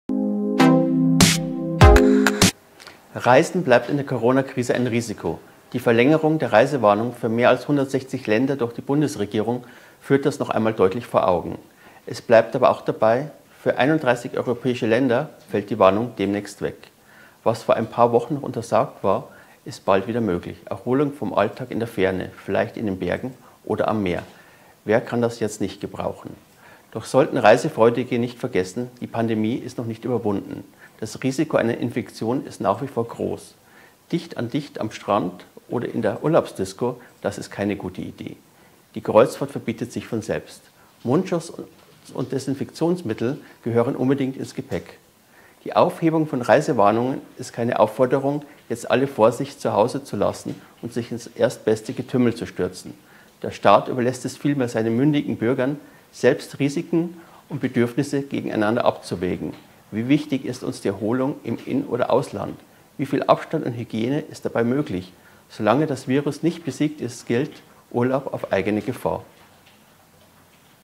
60 Sekunden zu Urlaub in Corona-Zeiten
Für 31 europäische Länder fallen die Reisewarnungen der Bundesregierungen demnächst weg. Trotzdem bleibt Reisen in der Corona-Krise ein Risiko, sagt Markus Lohmüller, stellvertretender Ressortleiter unserer Politik- und Wirtschaft-Redaktion.